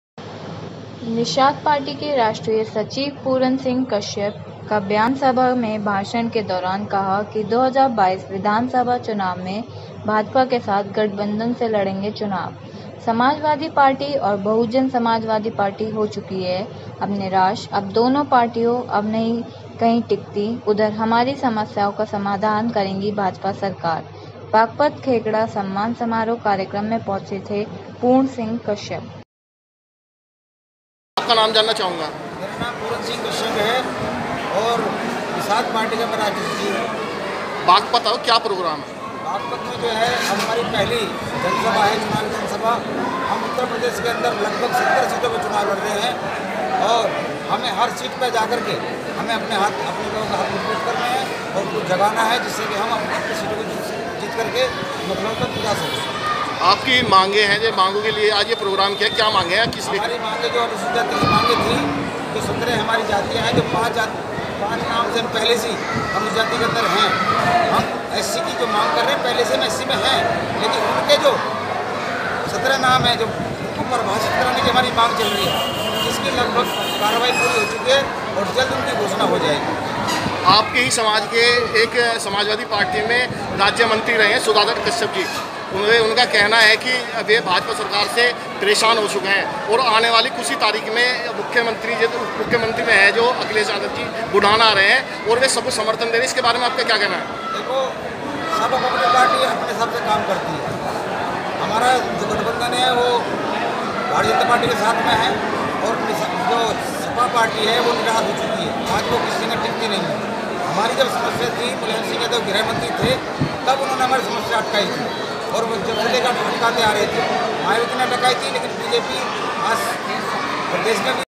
प्रदेश में छोटे दलों ने बढ़ाया भाजपा के साथ कदम।। निषाद पार्टी ने साथ चुनाव लड़ने का किया एलान।।
लाइव लोकवाणी न्यूज़ टीवी पर देखे ये बड़ी खबर,,,